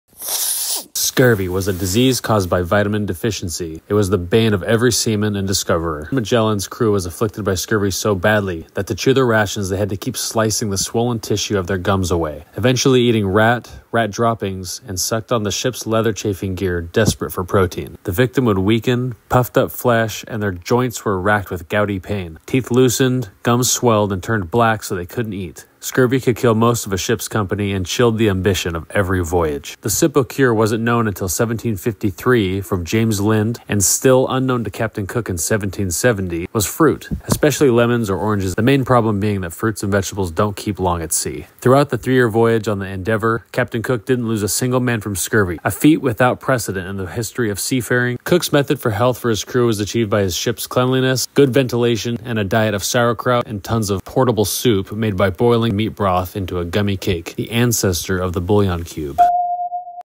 Scurvy On The Sea 1519-1770
Scurvy On The Sea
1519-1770

Scurvy a disease caused by Vitamin Deficiency.  Was the bane of every sea man and discoverer.

Ferdinand Magellan’s crew was afflicted by scurvy so badly that to chew their rations they had to keep slicing the swollen tissue of their gums away. Eventually eating rat, rat droppings, and sucked on the ships leather chafing-gear, desperate for protein.

The victim would weaken, puffy flesh and their joints were wracked with gouty pain. Teeth loosened, gums swelled and turned black so they couldn’t eat. Scurvy could kill most of a ships company and chilled the ambition of every voyage.

The simple cure which wasn’t known until 1753 by James Lind, and still unknown to Captain Cook in 1770, was fruit, especially lemons or oranges and green vegetables. the main problem being that fruits and vegetables don’t keep long at sea.

Throughout the 3 year voyage on the Endeavor, Captain Cook didn’t lose a single man from scurvy, a feat without precedent in the history of seafaring, especially in a time when longitude errors couldn’t be checked.

Cook’s method for health for his crew was achieved by his ships cleanliness, good ventilation and a diet of sauerkraut, malt, and tons of “portable soup”, made by boiling down meat broth into a gummy cake, the ancestor of the bullion cube.